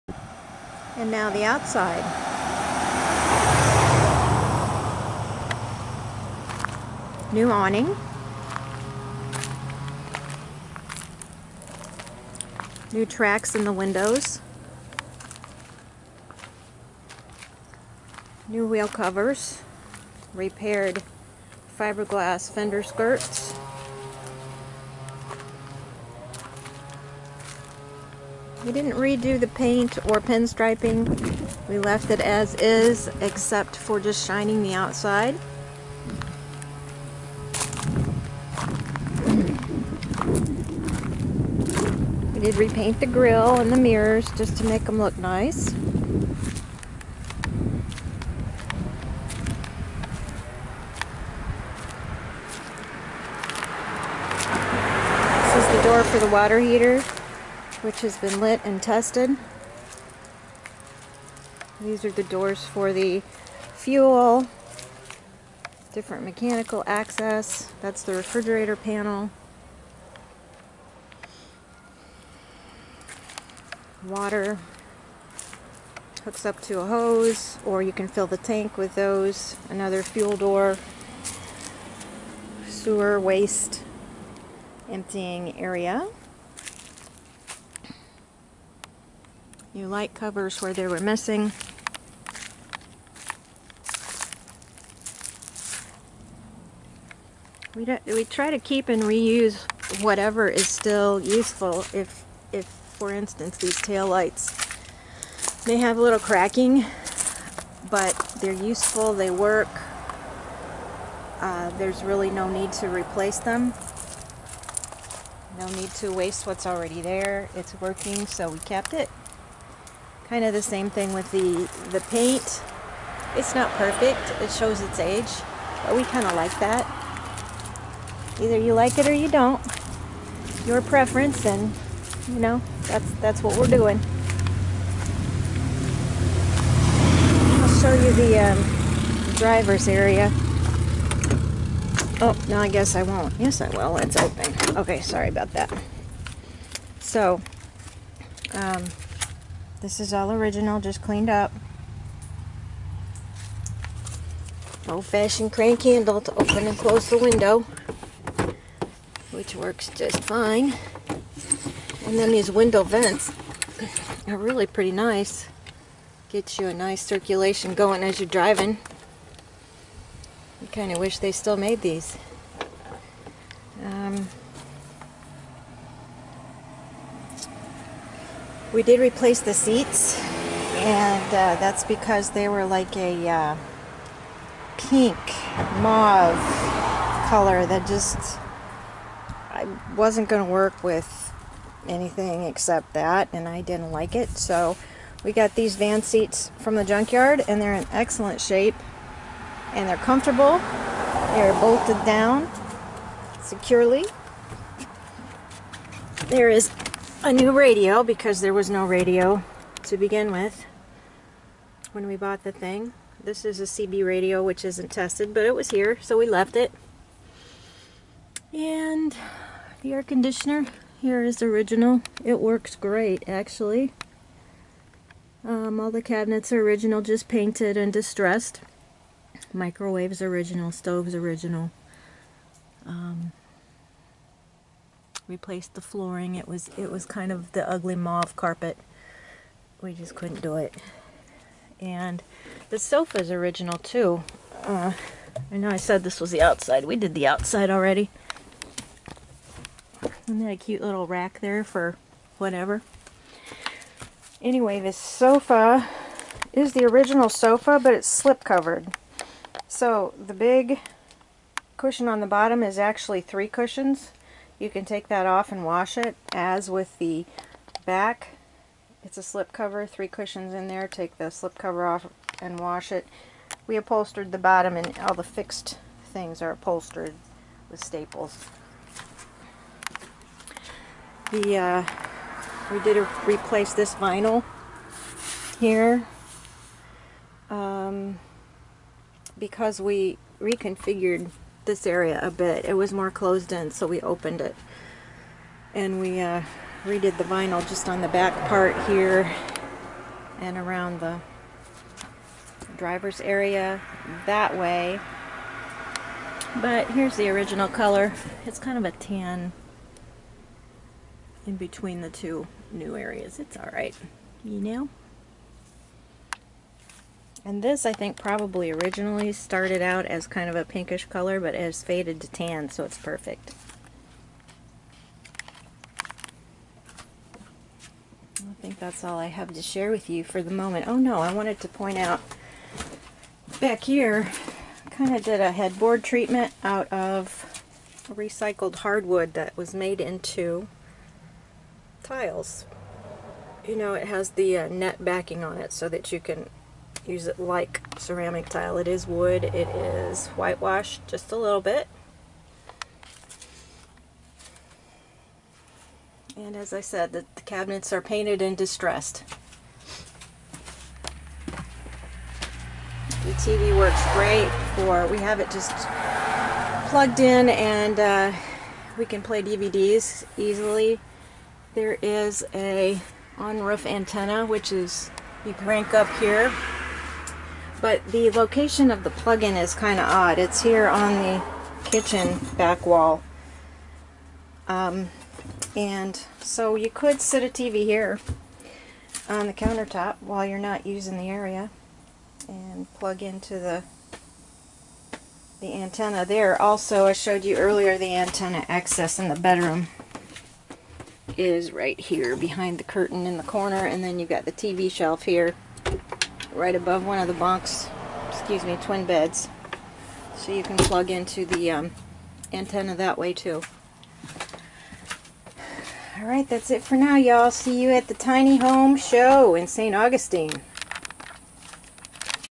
1986 Holiday Rambler Monitor Finished Exterior
Finished Monte's Exterior and Revisited Interior